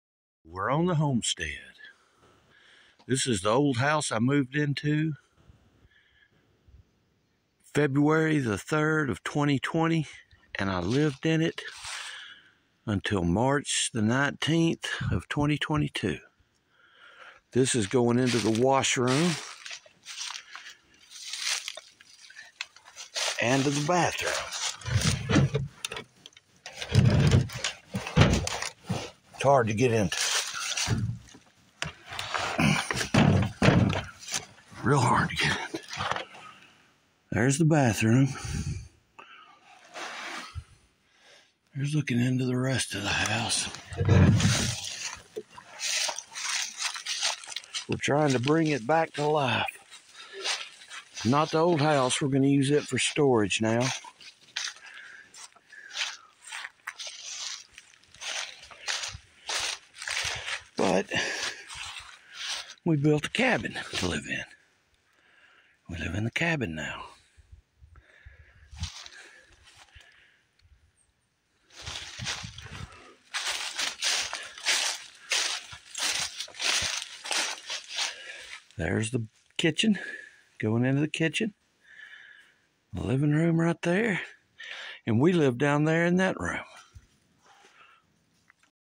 Where I lived for 2 yrs 1 month and 16 days. 🏡
No place like home. The old Homestead built in 1925, I lived here from 2-3-2020 till I moved into cabin on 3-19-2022. Making my dream come true on the farm with my dogs.